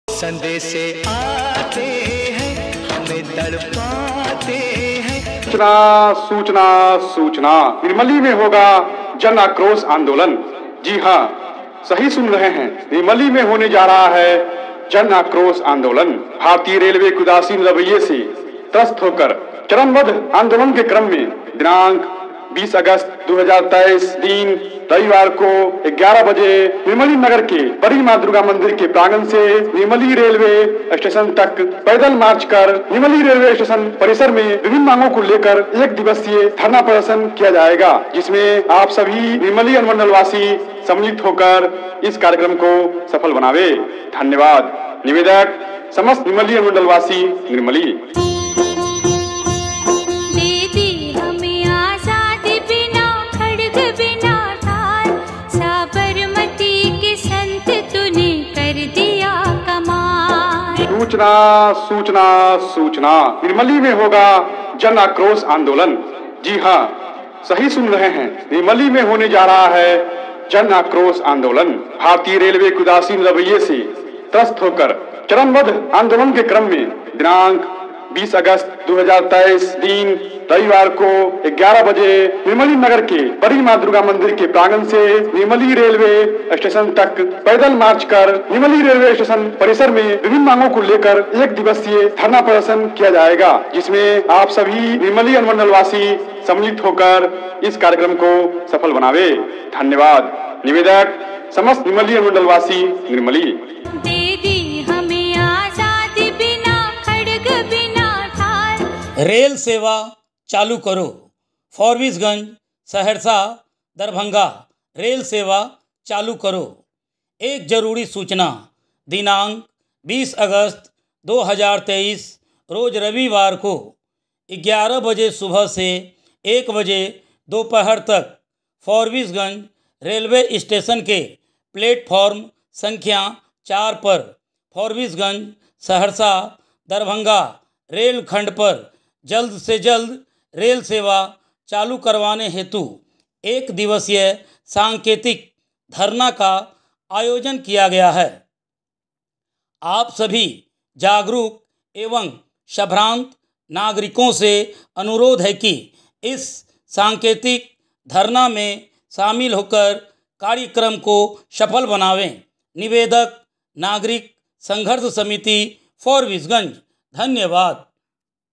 20 अगस्त रविवार को निर्मली फारबीसगंज में विशाल धरना-प्रदर्शन
20 अगस्त रविवार को निर्मली फारबीसगंज में विशाल धरना-प्रदर्शन @biharexpress24 biharexpress24 nirmali saraigadh raghopur forbesganj train ke liye dharna pardarshan